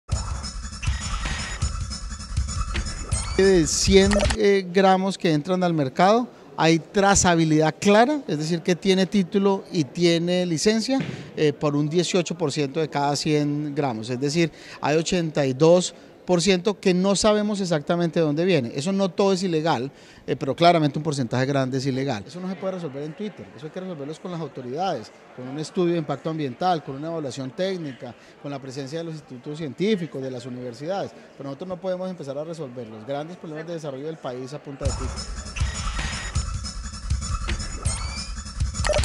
Gobierno desconoce procedencia del 82% del oro que se extrae en el país
Así lo dio a conocer el ministro de minas y energía, quien hizo un llamado a implementar las buenas prácticas mineras para combatir la ilegalidad.